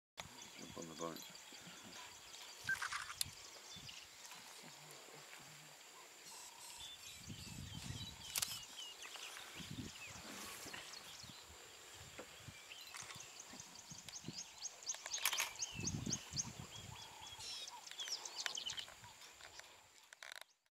Must-see 3d Leopard Encounter - Bring Your 3d Glasses!
Shangwa female leopard and son.
Remember when Youtube had random options to upload things in 3D?
This works with the old red and blue 3D glasses it is such old tech...

African Elephant (Loxodonta africana) t=1